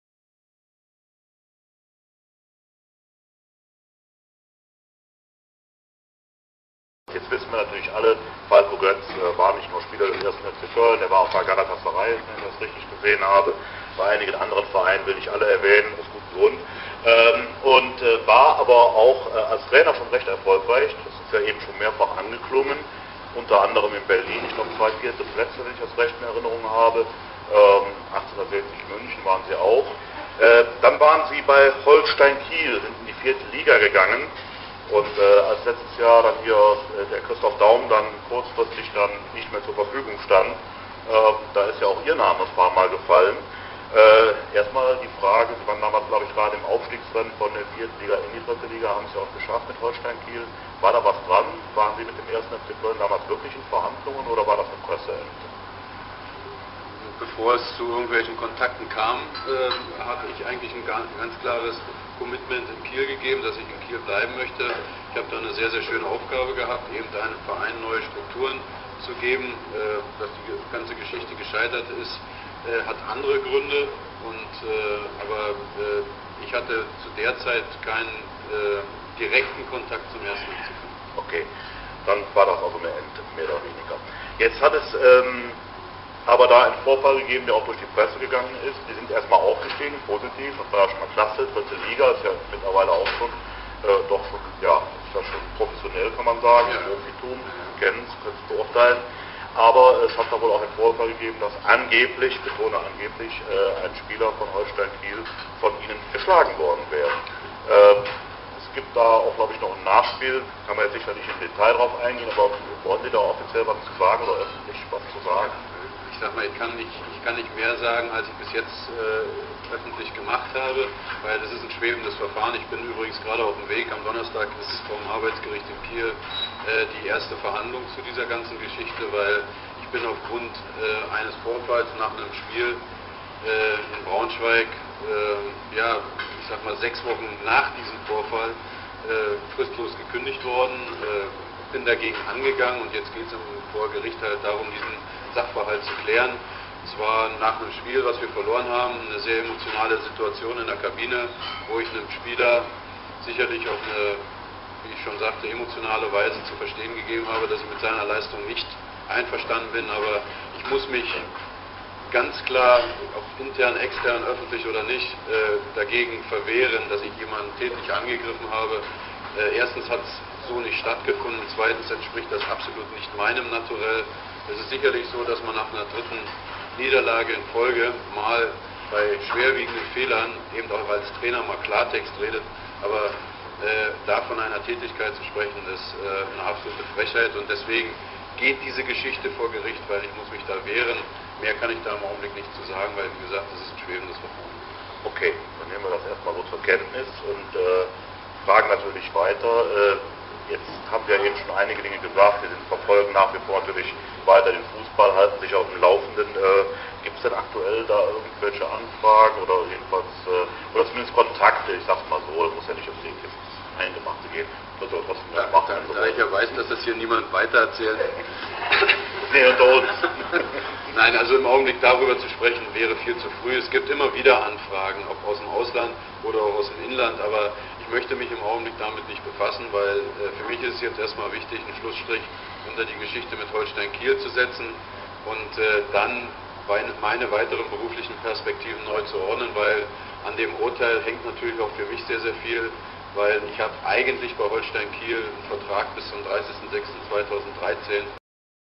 Falko Götz beim FC-Stammtisch-Talk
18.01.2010 - Falko Götz erklärt den Vorfall bei Holstein Kiel, der zu seiner Kündigung führte. Angeblich hat er einen Spieler geschlagen, ein Novum im deutschen Fußball, der die Medien beschäftigte. Gegen diese Behauptung wehrt sich der ehemalige Bundesligaspieler- und Trainer beim FC-Stammtisch Talk im Gaffel am Dom.